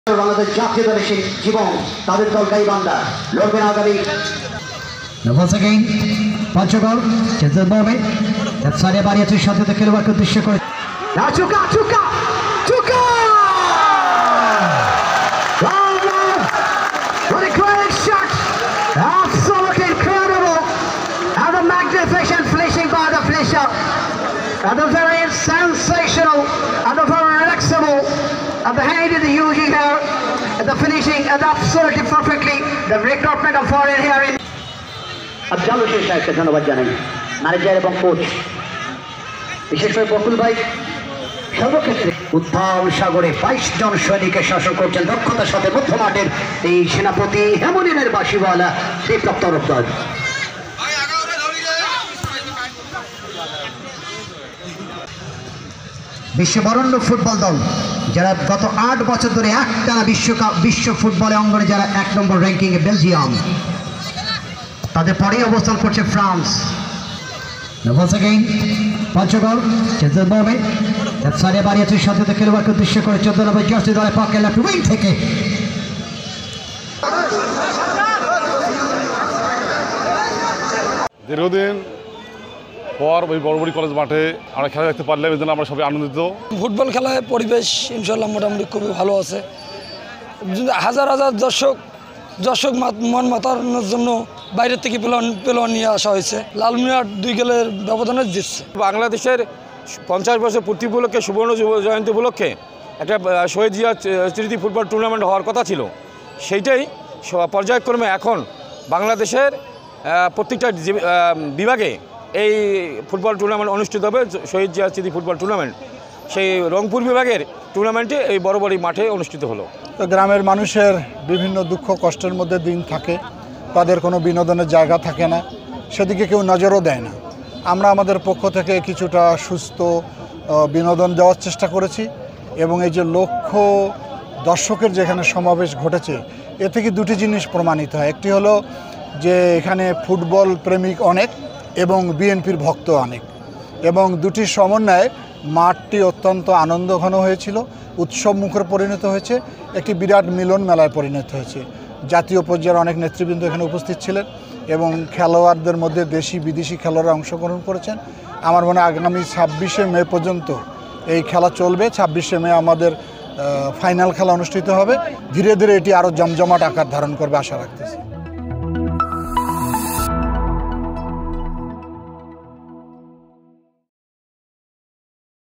এবার ফুটবল নিয়ে মাঠে নামল বিএনপি | BNP | Football | Lalmonirhat
রংপুর বিভাগের ৮ জেলা নিয়ে বিএনপি একটি ফুটবল টুর্নামেন্টের আয়োজন করেছে...